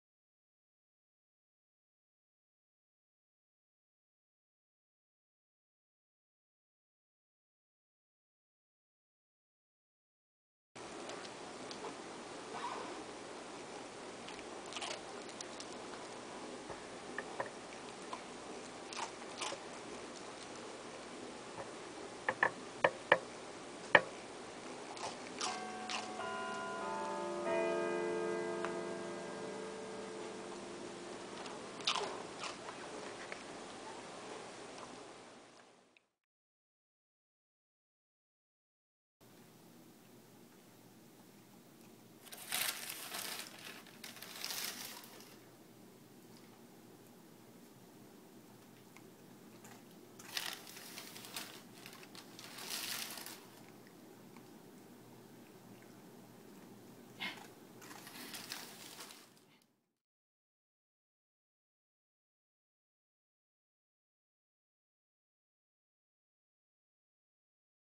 Browser The Whippet: Lazy & Hungry
Browser is laying on the sofa while Lisa feeds him, what a lazy little doggy!